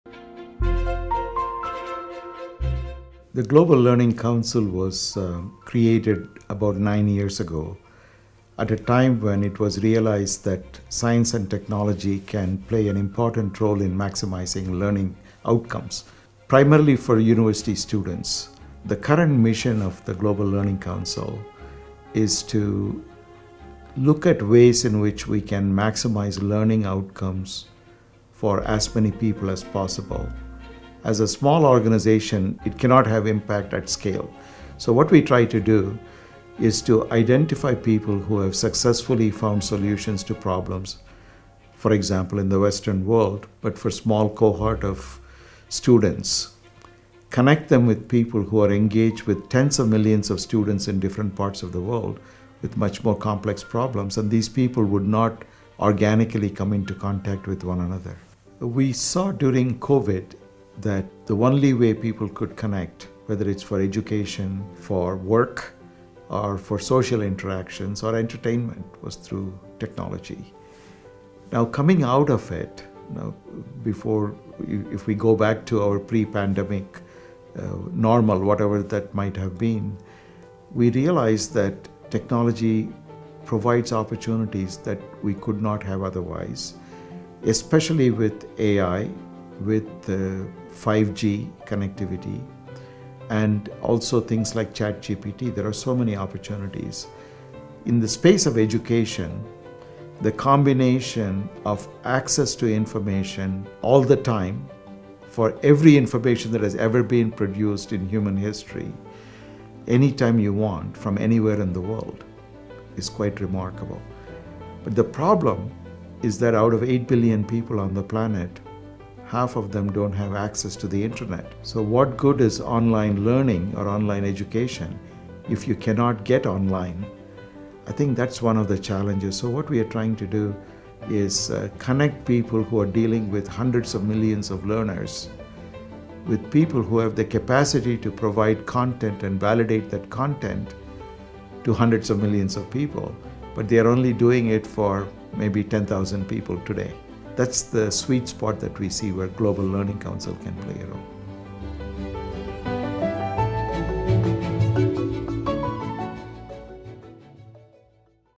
How Can We Scale Digital Solutions to Enhance Global Learning Outcomes?
Science and technology hold immense potential to enhance learning outcomes for students of all ages. However, the challenge remains: how can we leverage these innovative solutions to benefit those who still lack access to the internet?

The Global Learning Council (GLC) aims to bridge this gap by connecting education pioneers with learners from around the globe, fostering collaboration to improve educational outcomes at scale. By creating networks that facilitate access to information and shared resources, the GLC empowers educators and students alike, ensuring that no one is left behind in the digital age.

In this insightful session, GLC founder Subra Suresh discusses the organization’s mission to promote equitable access to education and the transformative role that data validation and systems thinking play in this effort. By harnessing the power of technology, the GLC is paving the way for a more inclusive educational landscape.

Join us to learn how innovative solutions can be applied to create meaningful change in the educational sphere, benefiting communities worldwide.